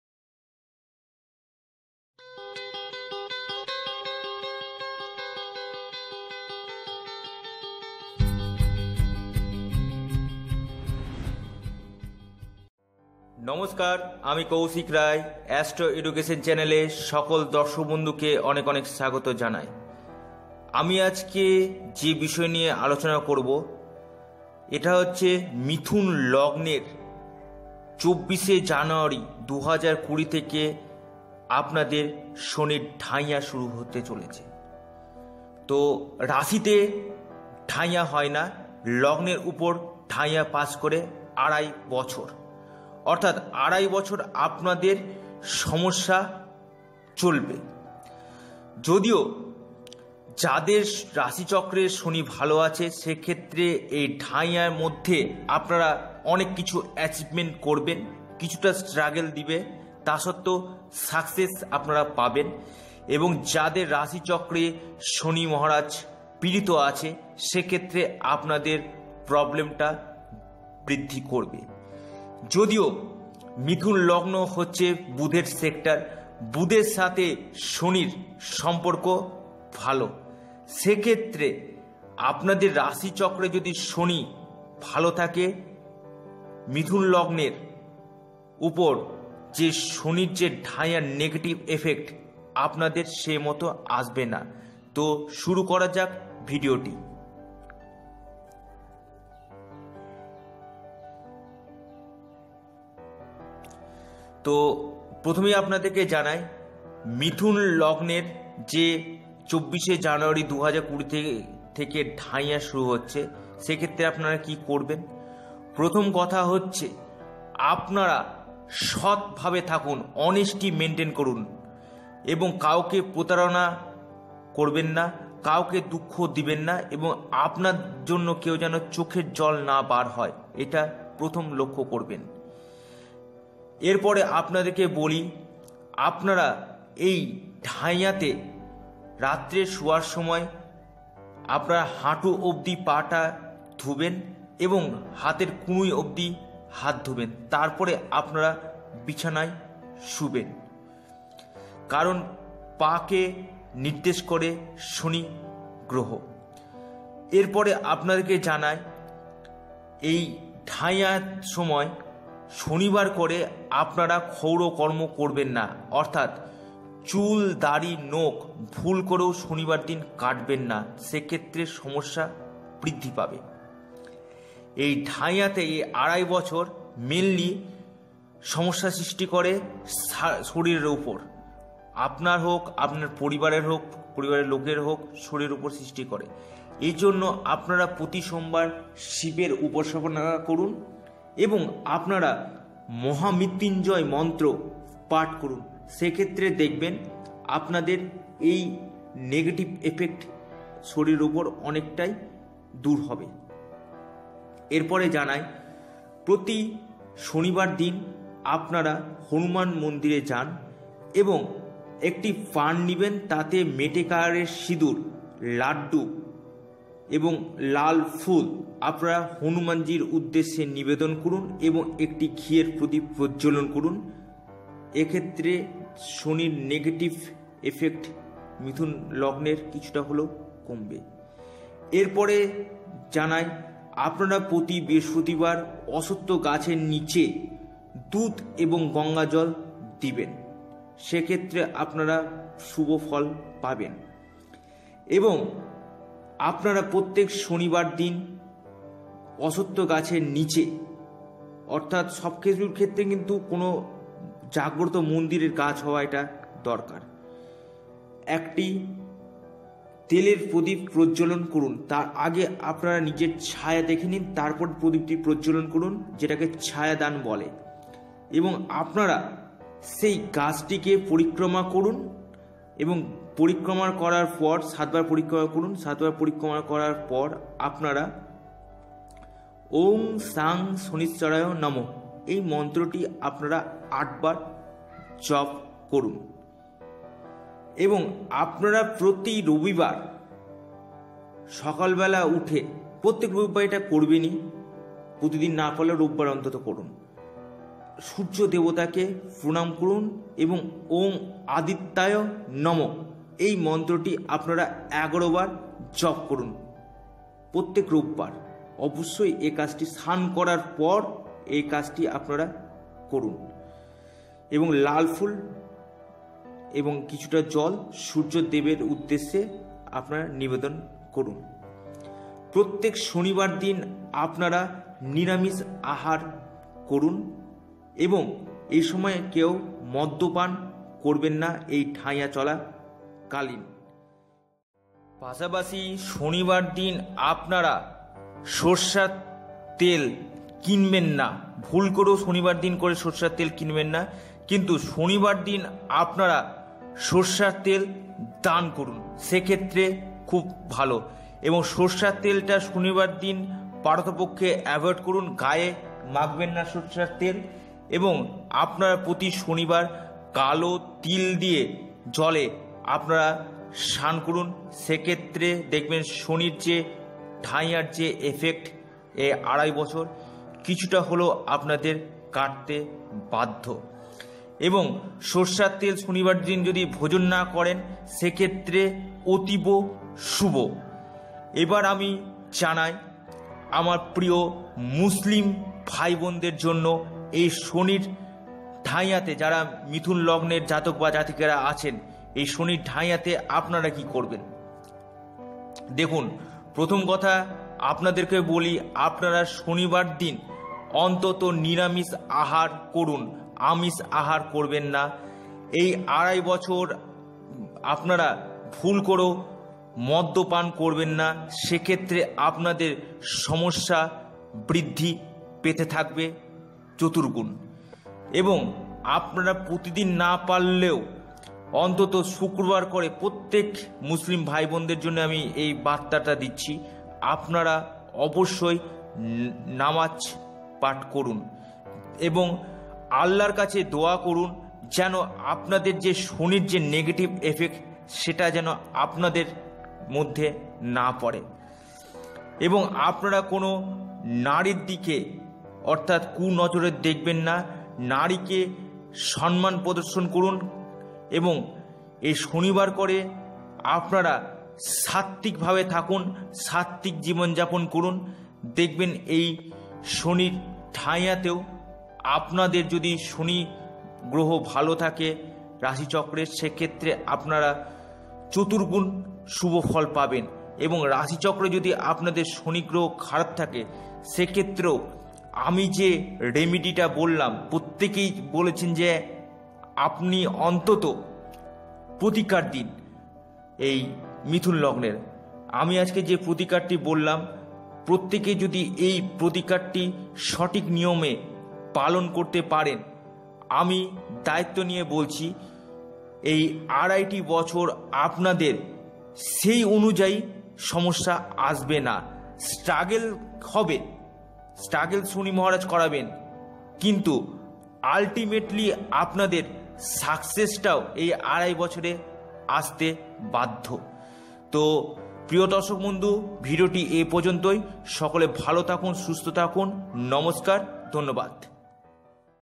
মিথুন লগ্নের ঢাইয়ার প্রতিকার!
মিথুন লগ্নে ঢাইয়ার প্রতিকার এই বিষয় নিয়ে বিস্তারিত আলোচনা করা হয়েছে!